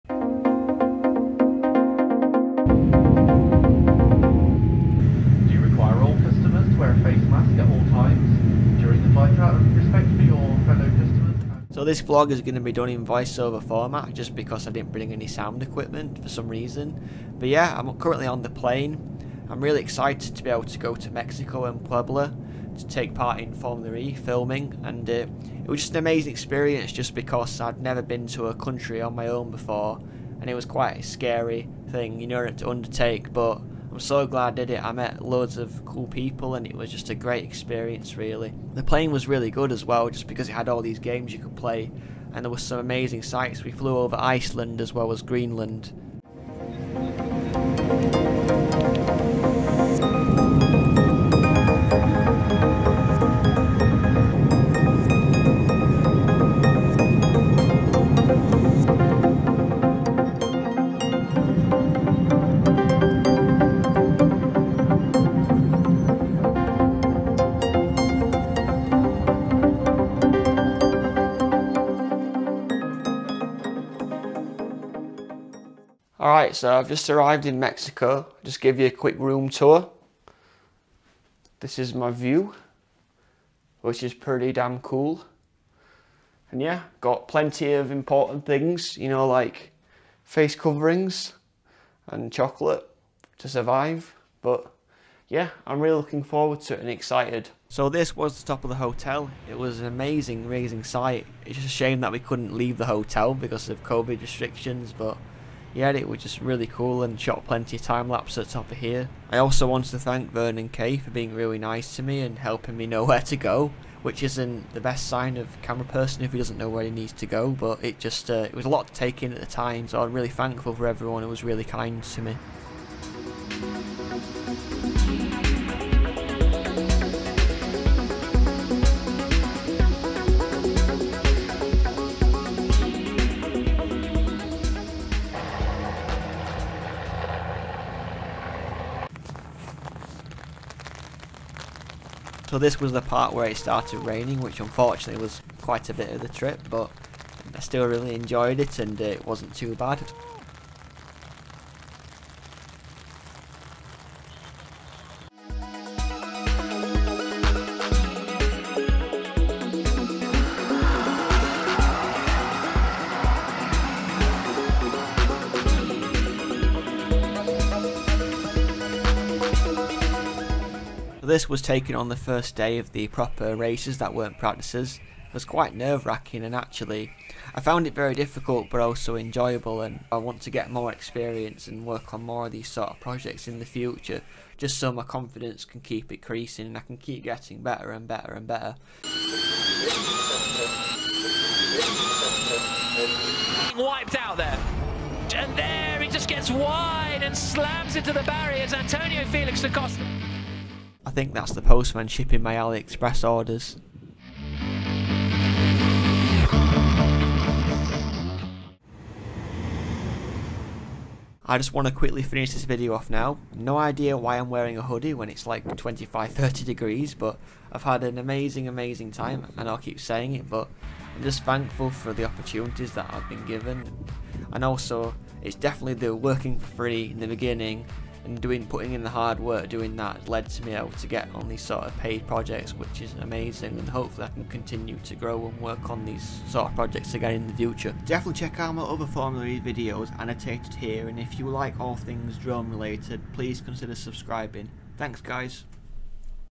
Formula E Mexico Puebla E-Prix VLOG
I want to say a massive thank you for opportunity to travel to Mexico during the pandemic to be part of an amazing filming crew for the Mexico Puebla E-Prix which was shown live on BT Sport and Eurosport. Despite the challenges of Covid-19 I had an amazing time that I will never forget!

The ABB FIA Formula E World Championship is the biggest single-seater motorsport championship for electric cars.

What Film Gear I Use